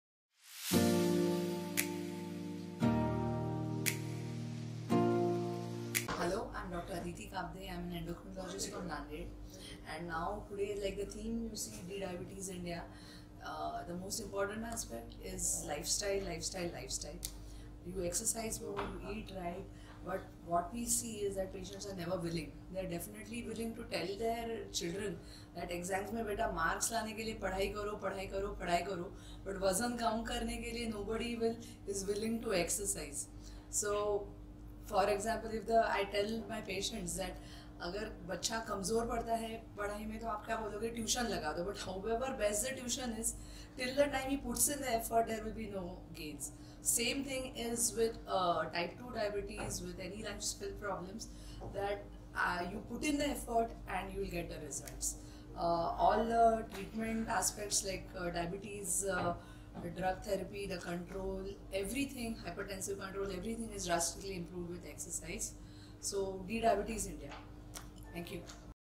How to Manage a Diabetic Lifestyle
Dr. Aditi Kabde (Endocrinologist) is talking about the "How to Manage a Diabetic Lifestyle".

Most often diabetes is very silent and there are no real symptoms even if the sugars are very high. In such cases, therefore, screening for this disease becomes very important especially in our country like India where the overall disease burden is very high.

This video session will help you understand Diabetes from its roots and take necessary actions to prevent yourself from it.

Share with your near and dear one and spread diabetes awareness to live Diabetes-free life.

Goal of De Diabetes India is Make India Diabetes Free.